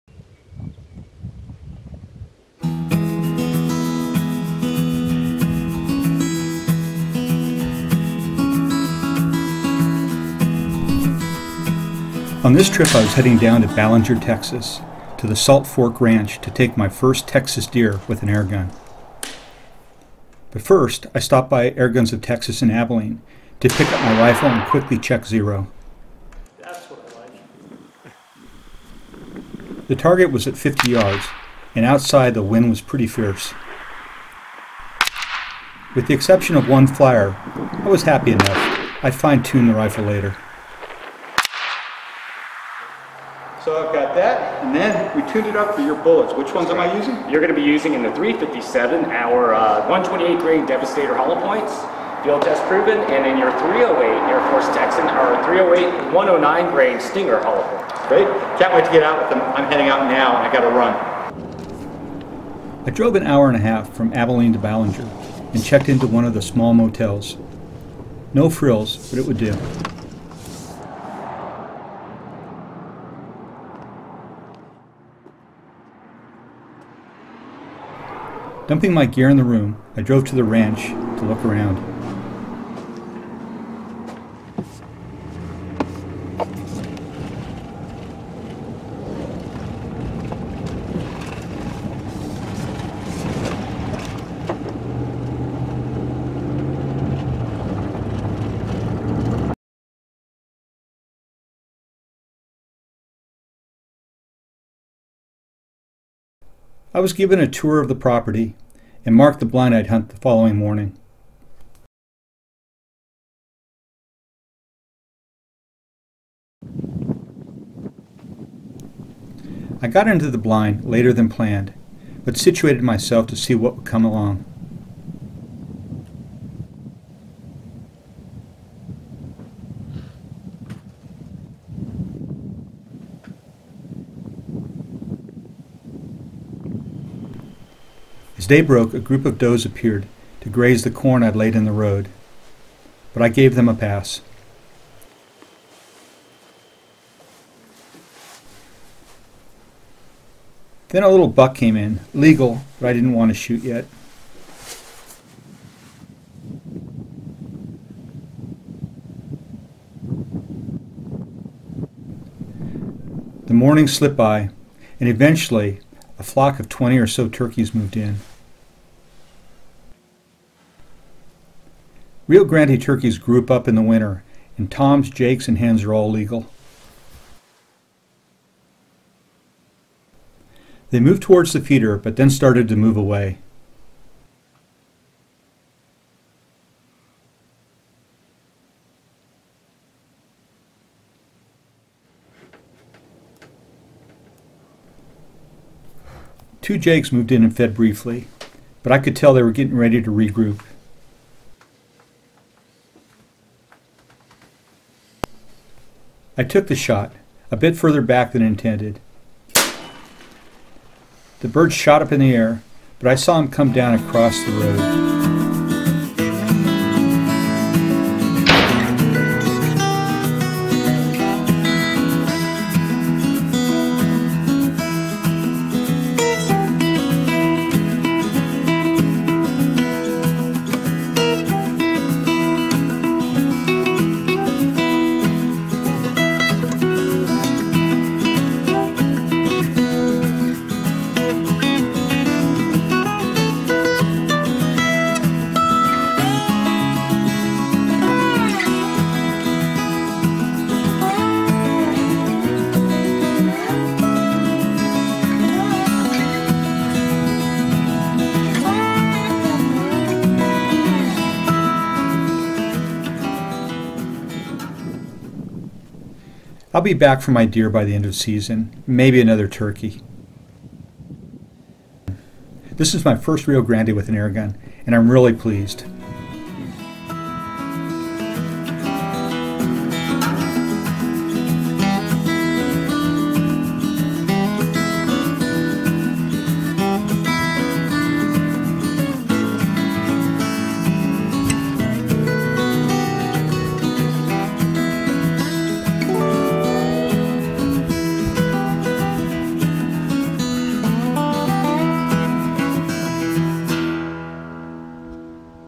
Airgun Hunting Rio Grande Turkey! The AirForce Texan .357
I take the The AirForce Texan .357 out to hunt Rio Grande Turkey, after my deer hunt.

California, Virginia, and Maryland (fall only) have allowed airguns for a few years. This year is the first time you can legally hunt Rio Grande turkey in Texas with an airgun. When I couldn't find the deer I wanted to shoot, my focus shifted to bagging a Rio Grande turkey. The deer and turkey seasons overlap, and I've read that probably more turkey are shot here with a deer rifle than a shotgun, and understand why. The rifle I'd intended to use for a whitetail was my AirForce Texan in .357. more than enough gun for my new quarry!

I pulled my shot on the bird as it was getting ready to take off, and hit a little further back than planned. The jake shot up in the air and dropped on the other side of the road. I'll be going back soon to score on my deer.

Texas changed their laws to allow all game with an airgun, and my goal is to get a deer, javalina, and turkey in this first airgun season!